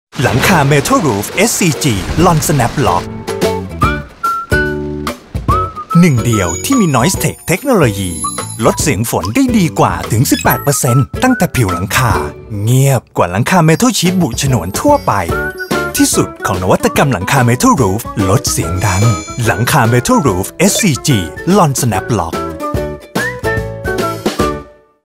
เมทัลชีทลดเสียงฝน ระบบหลังคา เมทัลรูฟ เอสซีจี รุ่น คอมฟอร์ท ลอน Snap Lock
ใหม่! ระบบหลังคา เมทัลรูฟ เอสซีจี รุ่น คอมฟอร์ท ลอน Snap Lock ด้วยเทคโนโลยี Noise TECH ที่มีการเคลือบสีคุณภาพสูงช่วยลดเสียงฝนตกกระทบบนหลังคาได้ดีกว่าสูงสุด 18%* (*รายละเอียดคลิก

-ป้องกันความร้อนได้มากกว่า ด้วยเทคโนโลยี HeatTECH ของฉนวนกันความร้อน เอสซีจี ช่วยลดอัตราการไหลเข้าของความร้อน เมื่อเทียบกับเมทัลชีทลอนทั่วไปที่ติดฉนวน ได้ดีกว่าถึง 25%** (**รายละเอียด คลิก
-รูปแบบลอนเป็นเอกลักษณ์ เหมาะสำหรับบ้านที่มีความเท่ สมัยใหม่
-รับประกันสีสวยยาวนาน 12 ปี